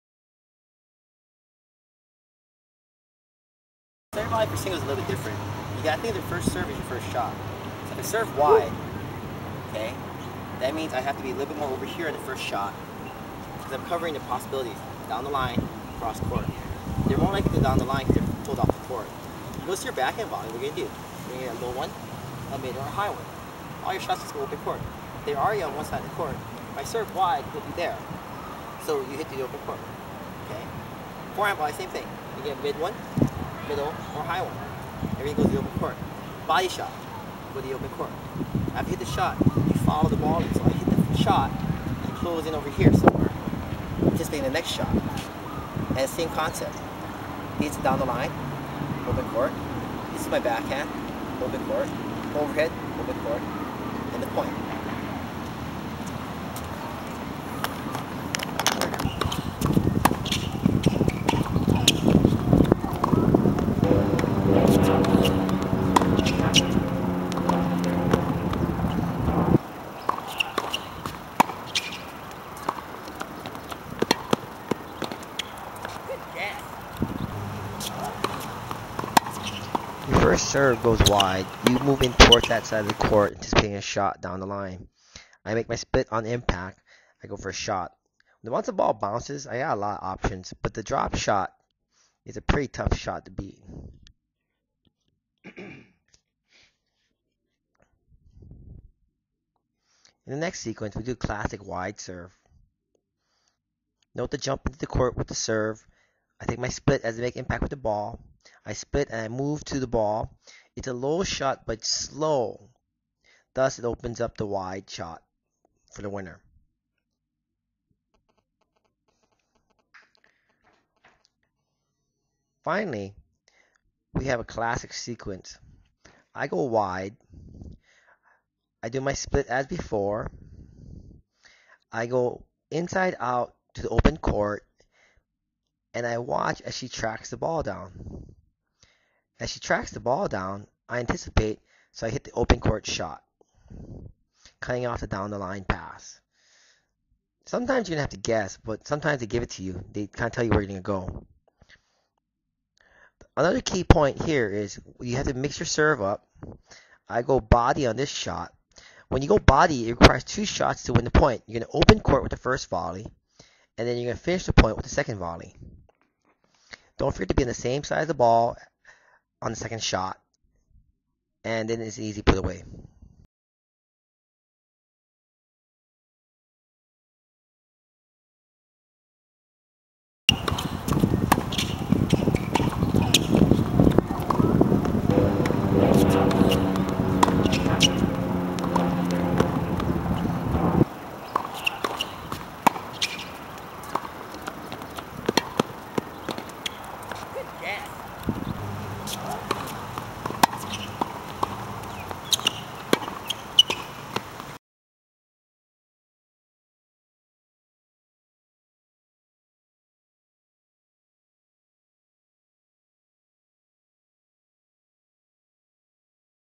How to Play Singles--serving and volleys plus positioning
shows how to serve and volley--it's all plays babies.